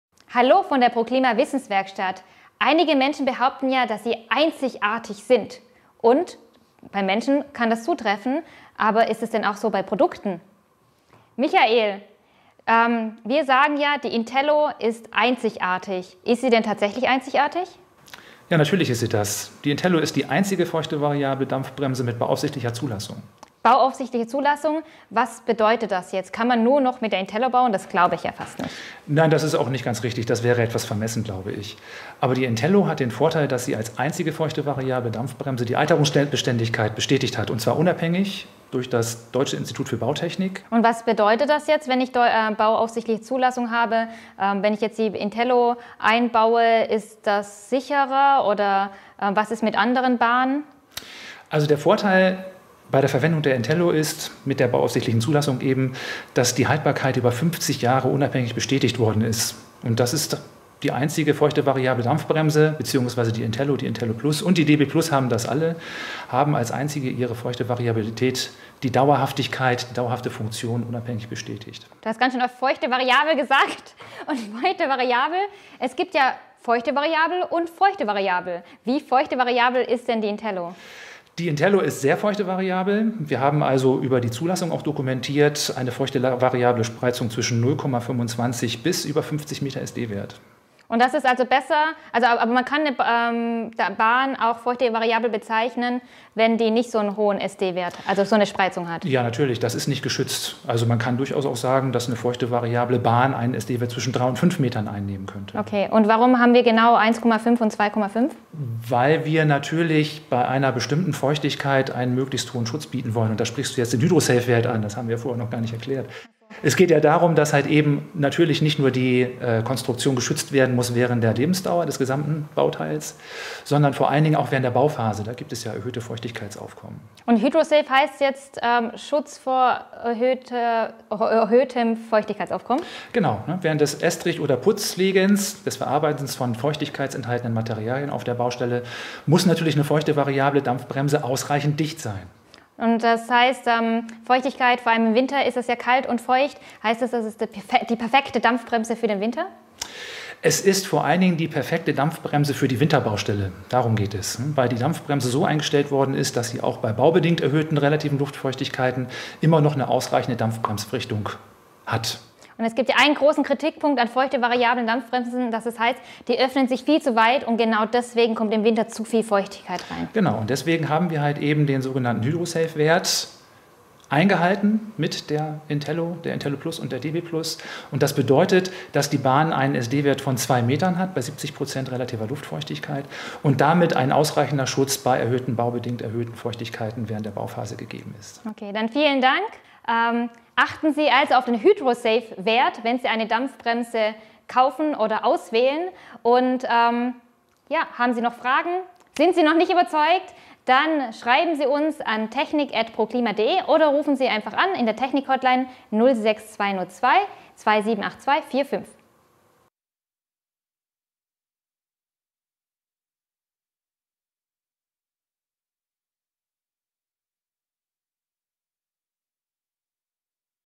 Winterbaustelle? Feuchtigkeit? Kein Problem mit INTELLO
pro clima INTELLO ist die einzige feuchtevariable Dampfbremse mit bauaufsichtlicher Zulassung. Es gibt einige feuchtevariable Dampbremsen - wo hier die Unterschiede liegen erkärt Michael Förster (Leiter pro clima Anwendugnstechnik) im Video. Er erkärt auch, warum eine feuchtevariable Feuchtigkeit mit gutem Hydrosafe-Wert ideal gegen Feuchtigkeit auf der Baustelle ist.

Natürlich erklärt er auch, was Hydrosafe überhaupt bedeutet. Viel Spaß beim Schauen.